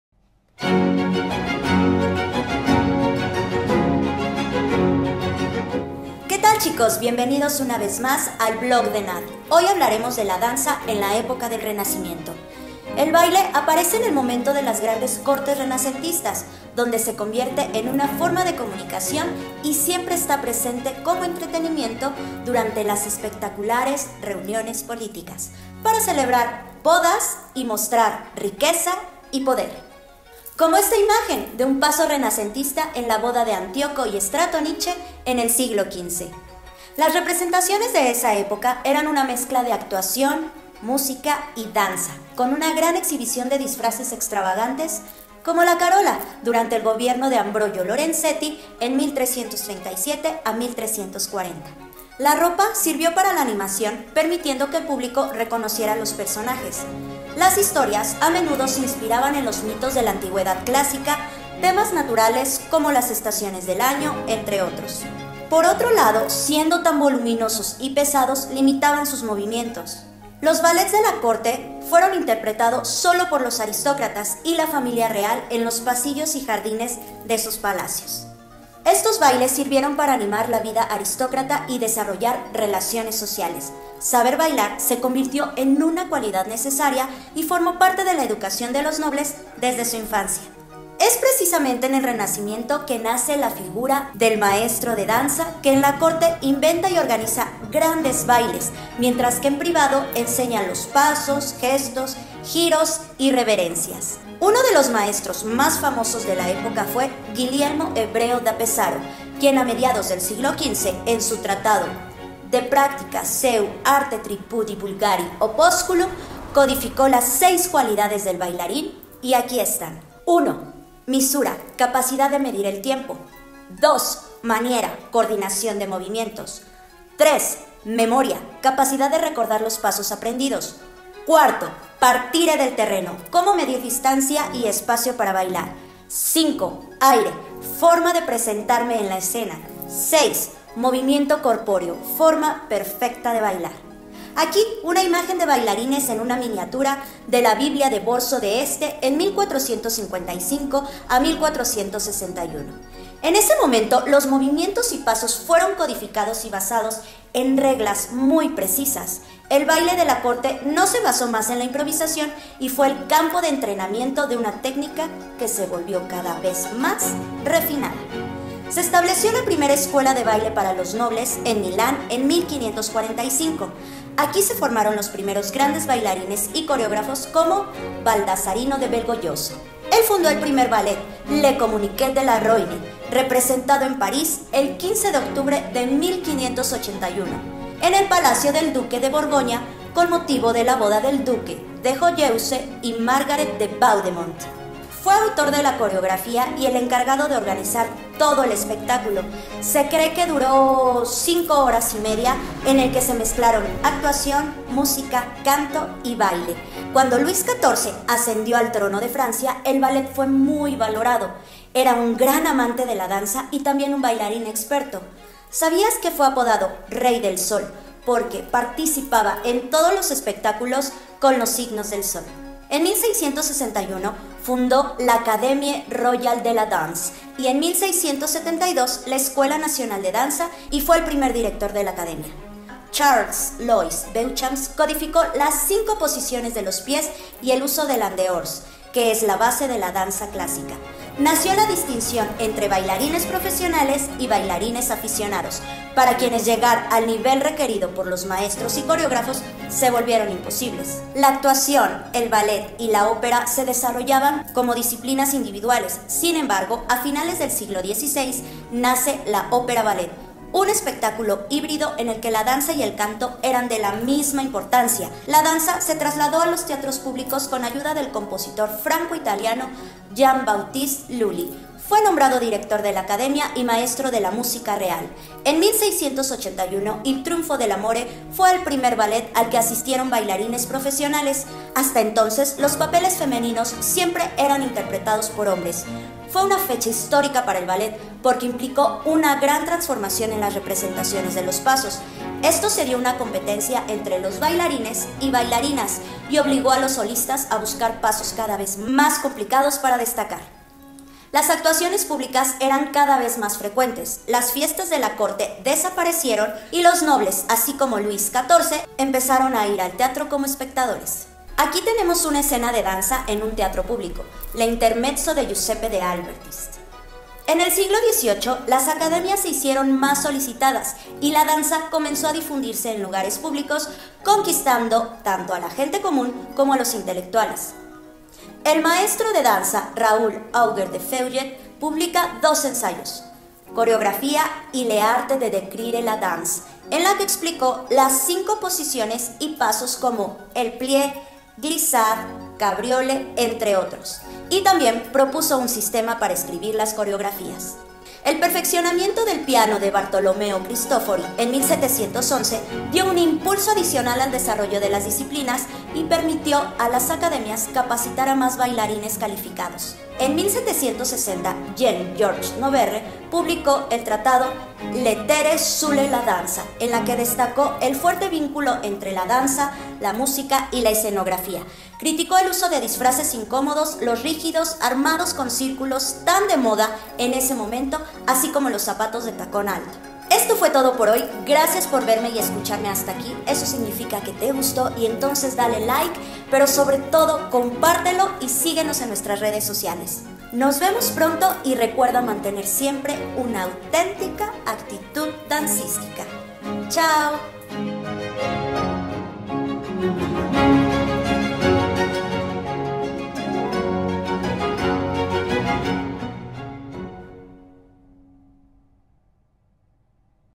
ENAD- El Vlog 03- Danza en el Renacimiento
En el ElVlog de hoy aprenderán el origen del Ballet, como paso de ser un baile para aristócratas, a gente común. Cuáles son las cualidades de un bailarín, El rey del sol Luis XIV. y más.

@enadanza instagram y facebook

Gracias!!!
Auténtica Actitud Dancística